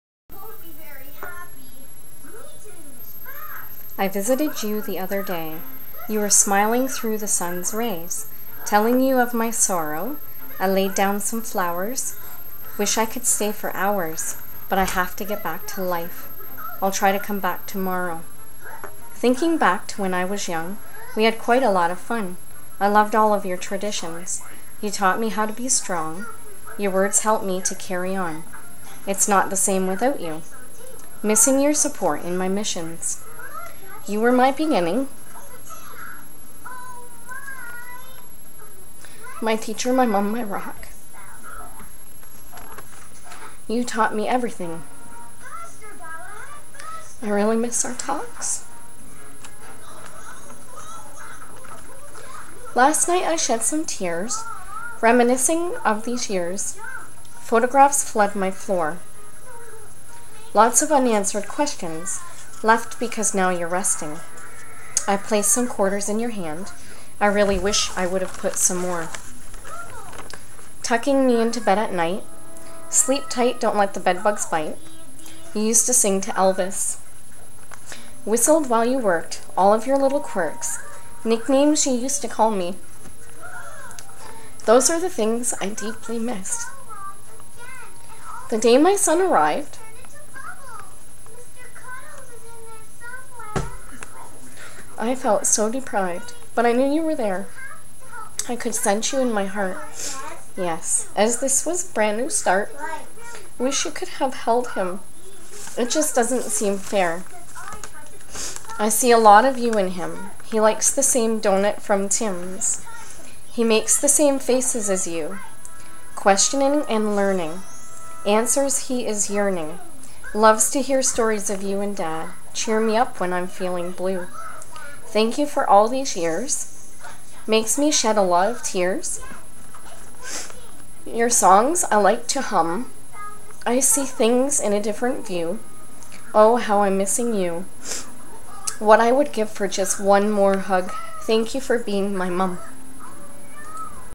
Mompoem
A poem I wrote for my Mom last year, missing her and thinking of all the good memories. Wish she could have held my son when he was born along with watching him grow. This was difficult for me to record.